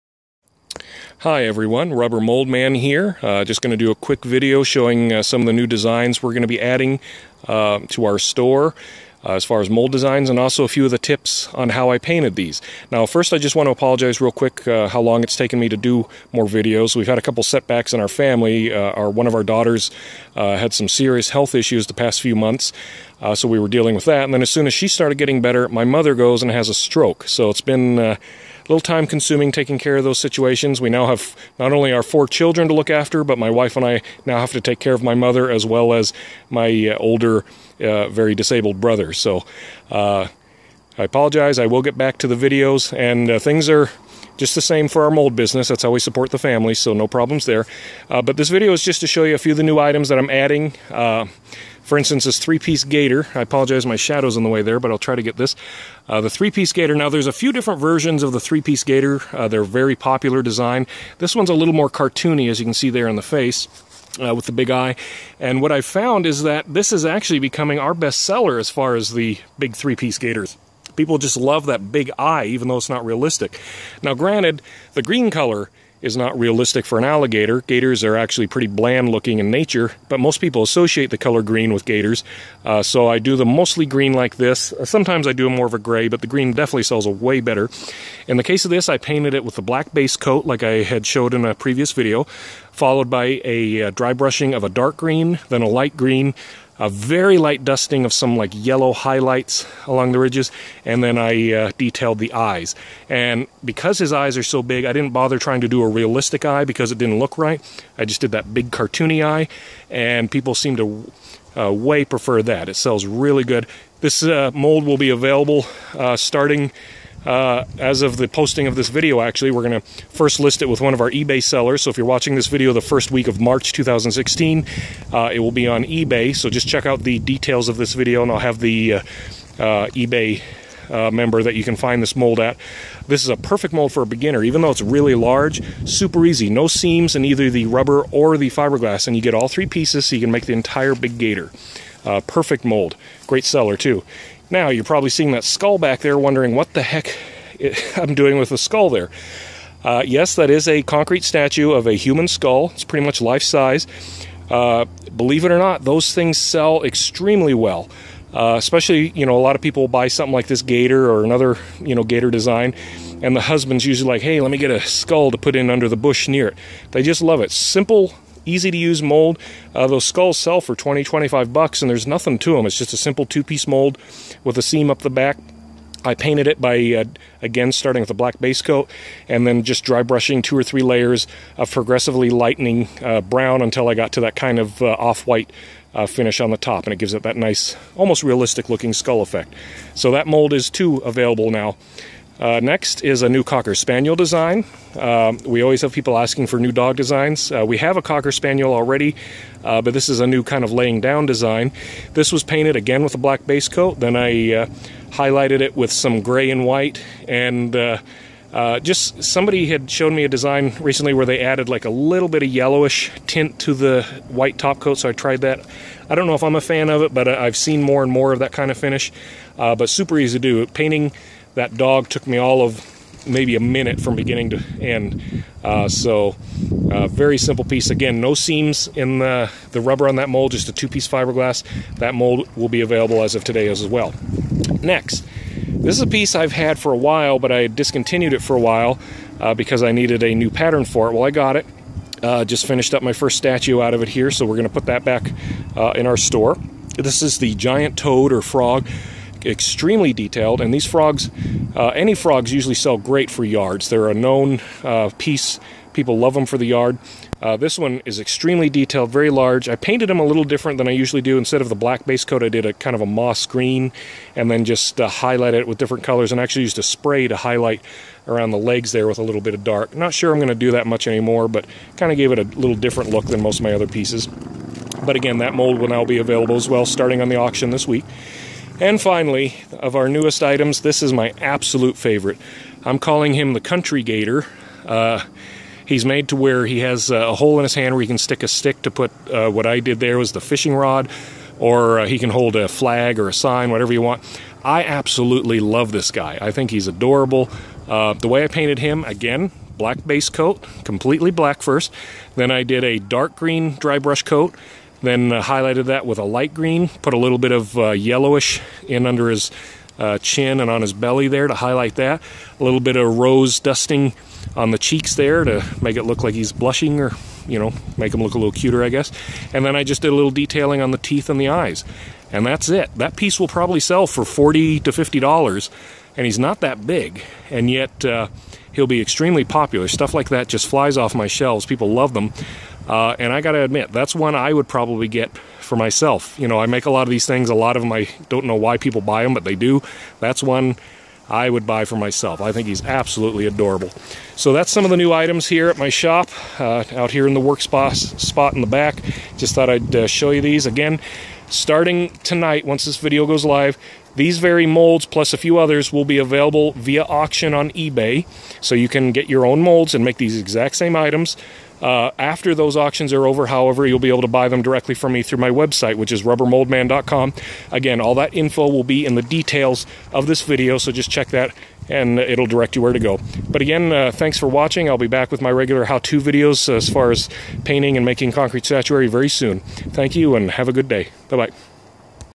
New mold/statue designs plus painting tips
These are a few new mold designs that I am now offering for public purchase. Starting the first week of March 2016 these molds along with a few others will be on auction from eBay member caservicesandtradesinc. After that they will be available at my website rubbermoldman.com